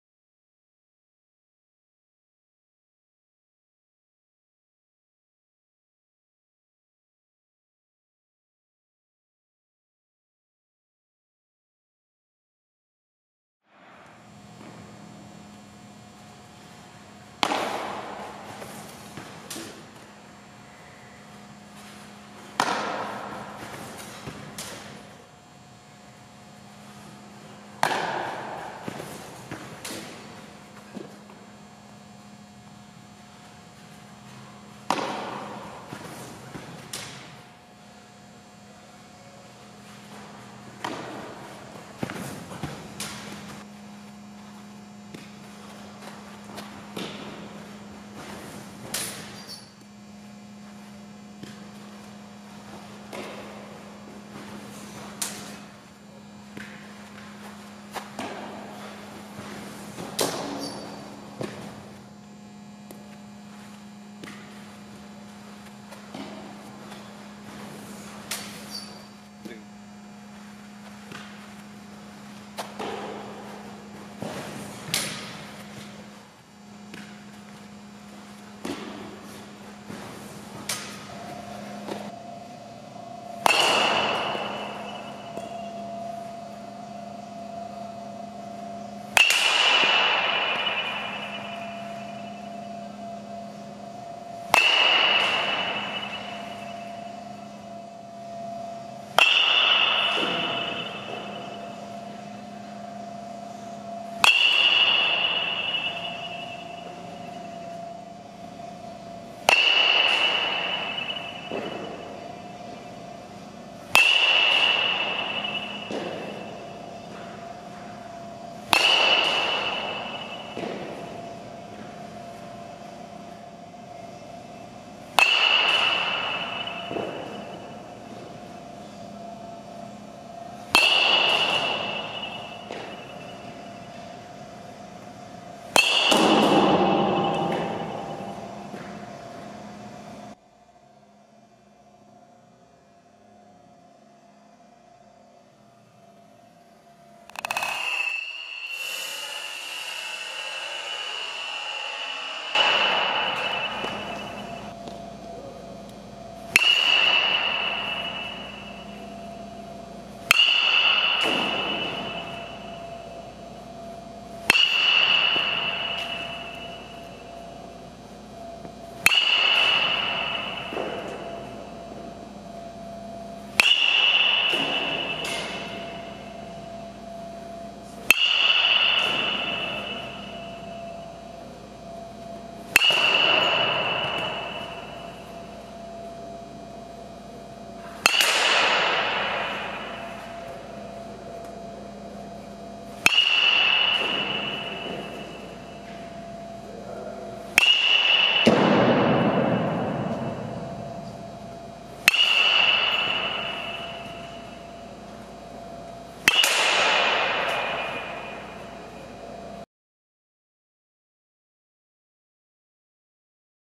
Brennan Reback (Fielding & Hitting)
Brennan Repack (Class of 2017)

Position: OF
Height: 6'0"
Weight: 170
B/T: L/L

School: Vernon Hills (IL)
Club Team: Slammers (IL)